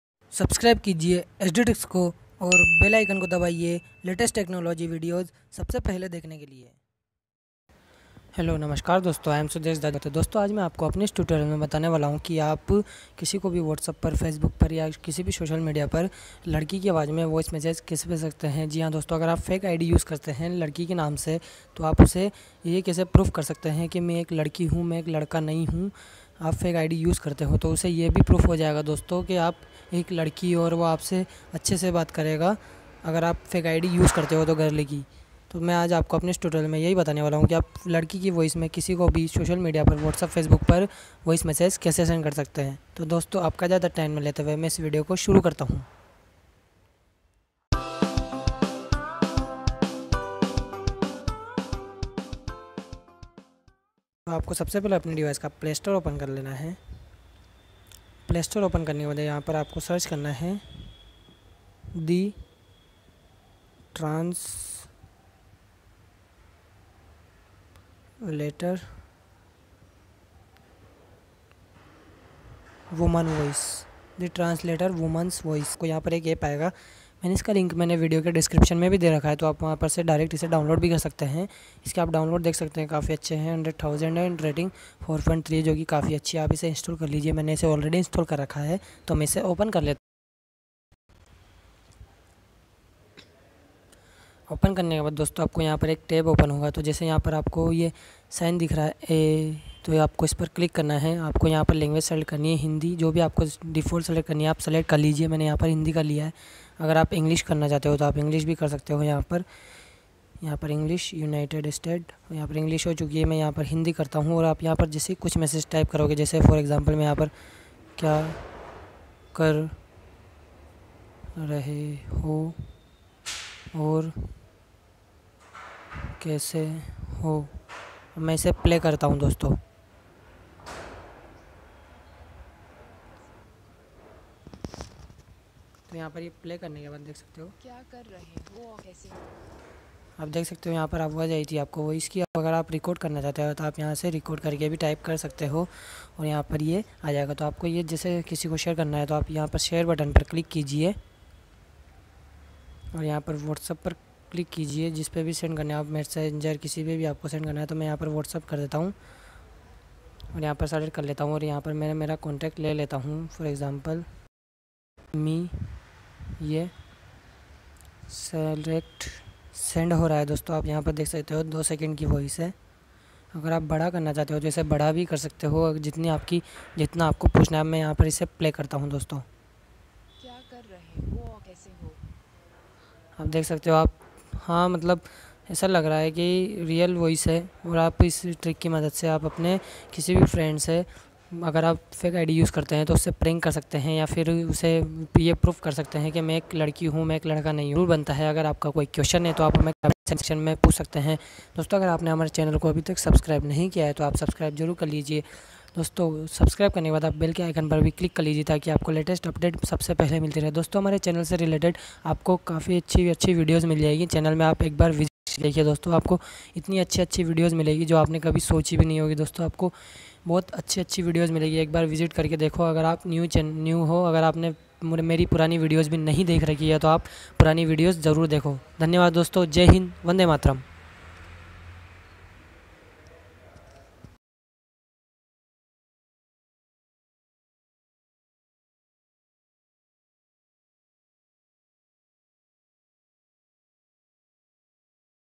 How to Send Voice Massage in Girl's Voice On WhatsApp & Facebook
Namaskaar dosto aaj main aapko is video m bataunga ki aap kisi ko bhi girl ki voice me massage kaise send kar sakte hai WhatsApp or facebook par to jaanane ke liye last tak ye video jarur dekhe.

thanx for watching guyz.